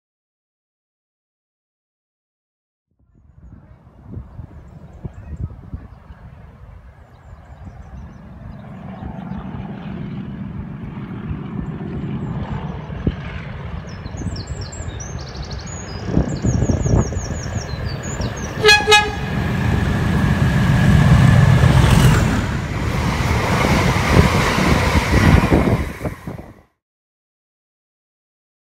Europhoenix 37800 through Longfield station 18th March 2017
A rare day off fo me and whilst sitting in the cafe i remembered the unit transfer was due through, as the station is across the road it would have been rude not to film it.